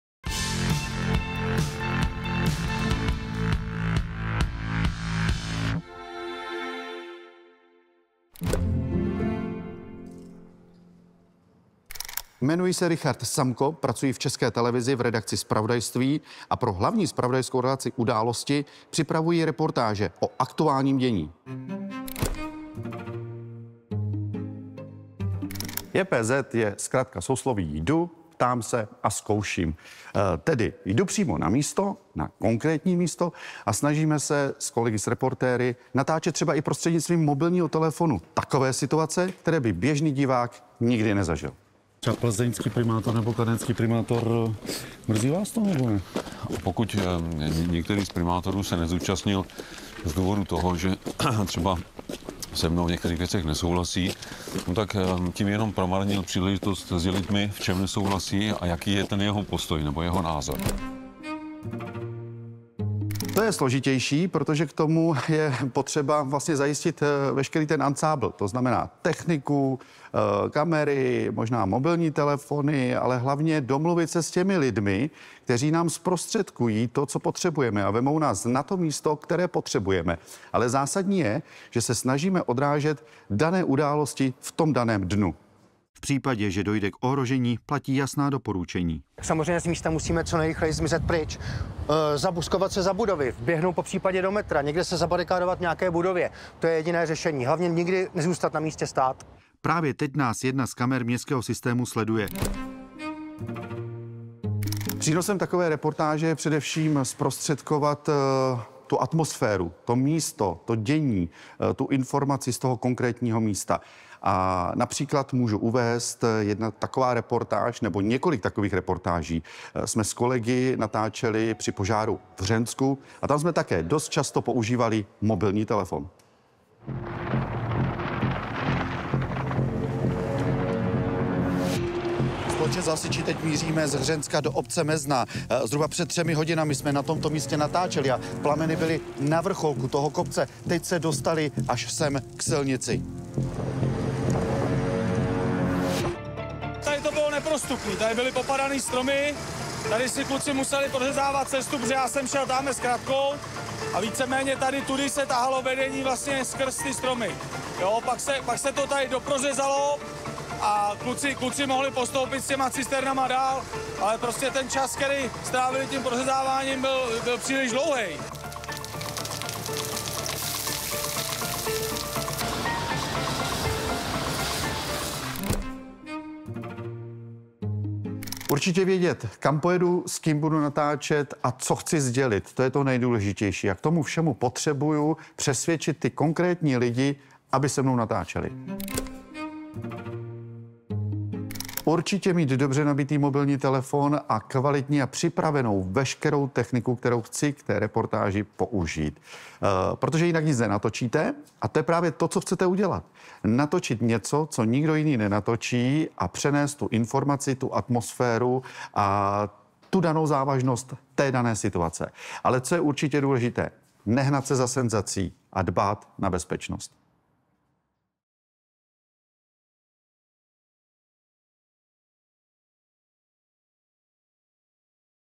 Reportéři na startu | Jak na reportáž typu „jdu, ptám se, zkouším”?
Redaktor ČT Richard Samko podrobněji představuje zpravodajský žánr JPZ, tedy „jdu, ptám se, zkouším“. Na co si dát pozor, když redaktor vyráží přímo do centra události? Jak hledat respondenty na místě? 🎤

Video vzniklo v rámci soutěže Reportéři na startu, kterou pořádá portál ČT edu.